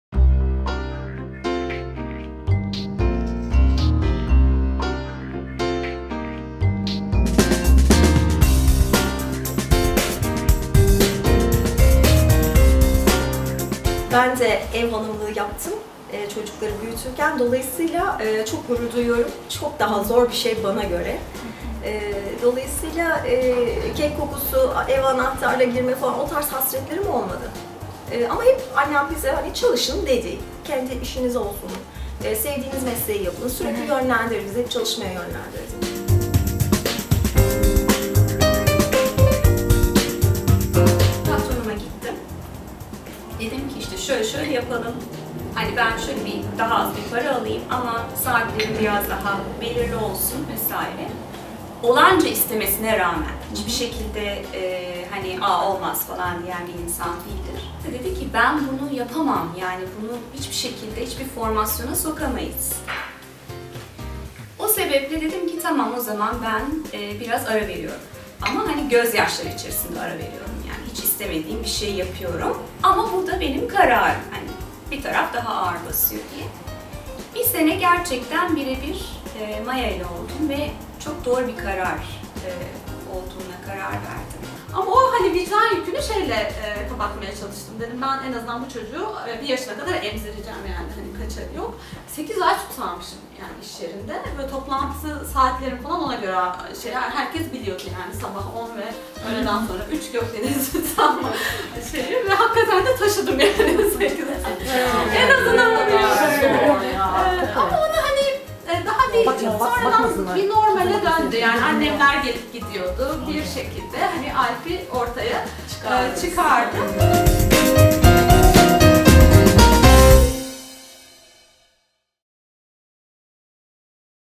YenidenBiz ile Çocuktan Sonra İşe Geri Dönmek
Kariyerine ara vermiş, iş hayatına dönmek isteyen eğitimli, deneyimli kadınlar için olan platform YenidenBiz ile tavsiyeevinde çok keyifli ve bilgilendirici bir sohbet deneyimi yaşadık.